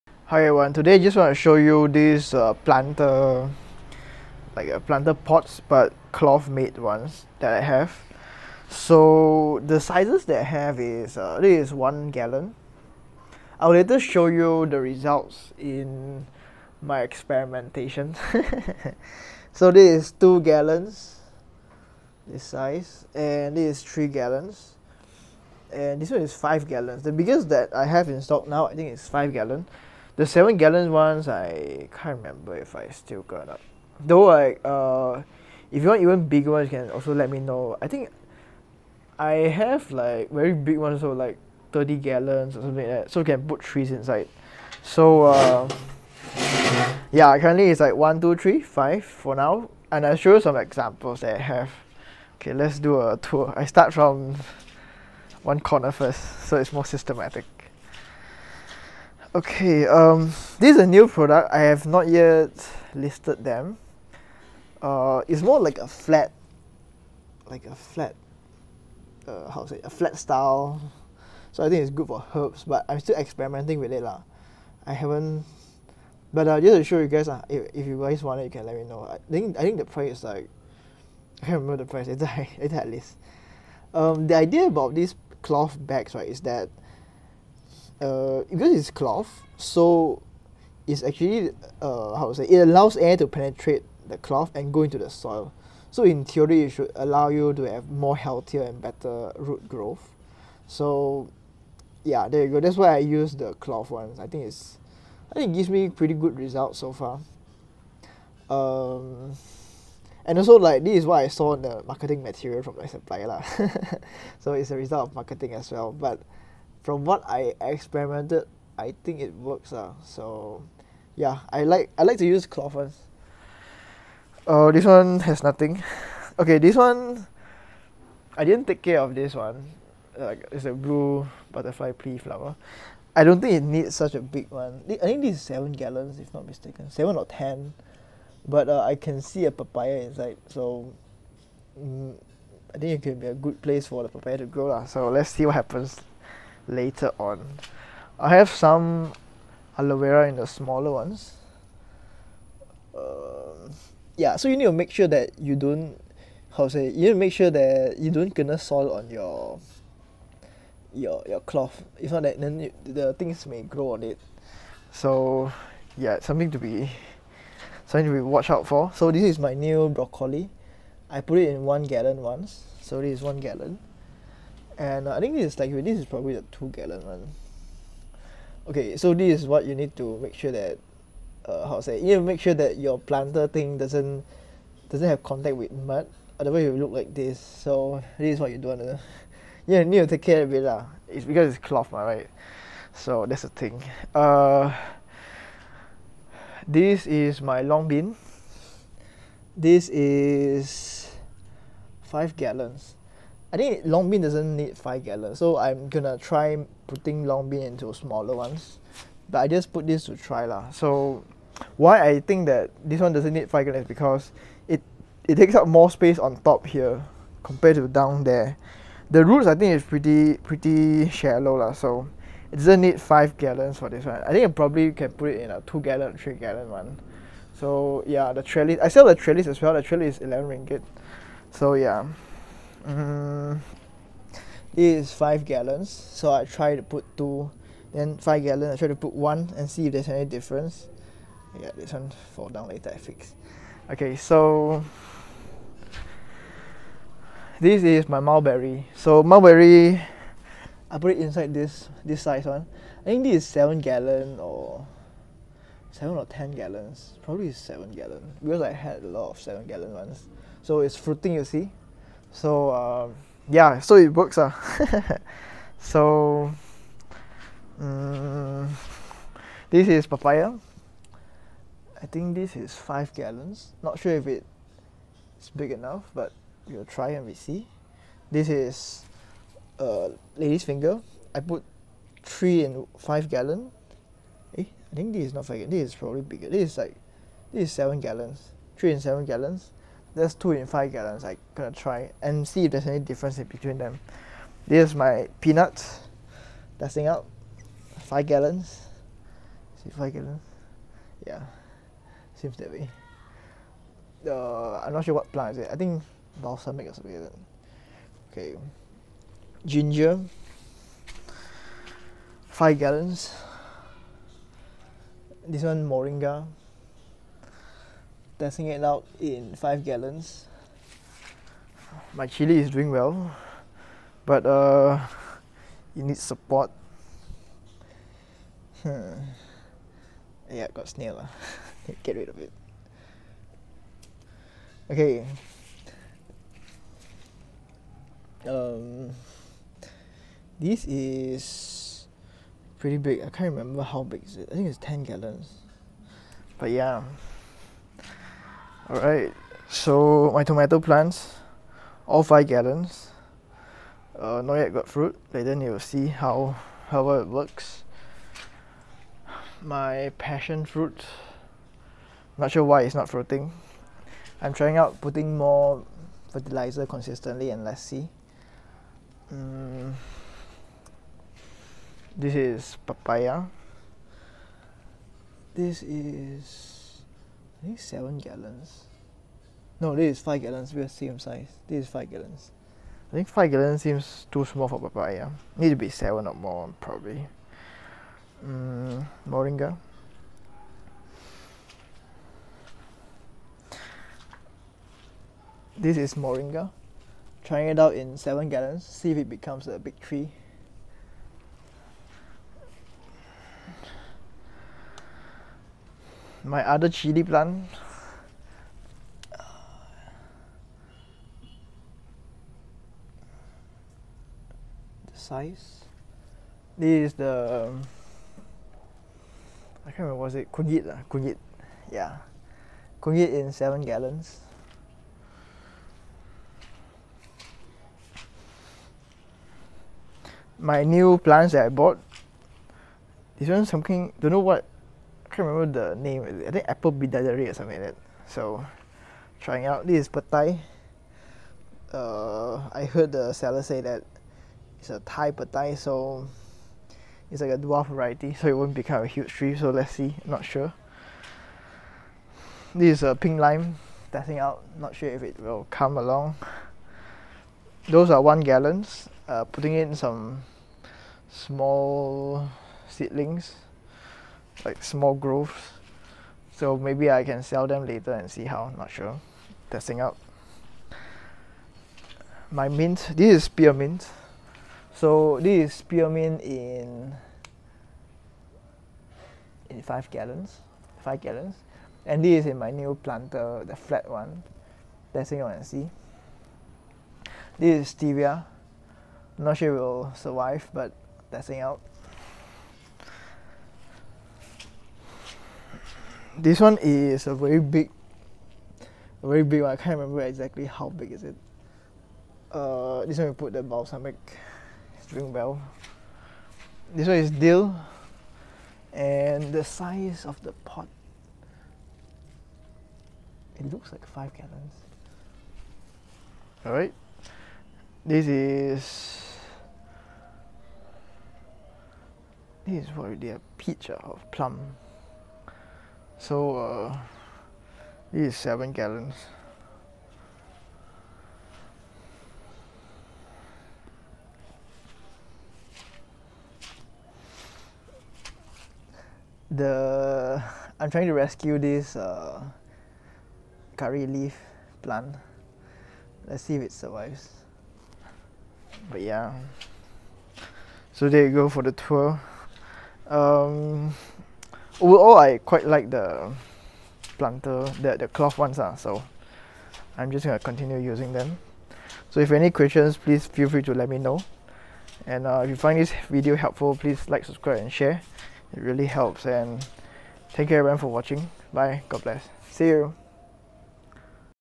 Grow Bags Demonstration ( Nonwoven Cloth Planter Pots )
In this video we demonstrate these Grow Bags ( Nonwoven Cloth Planter Pots )

In this video we demonstrate this item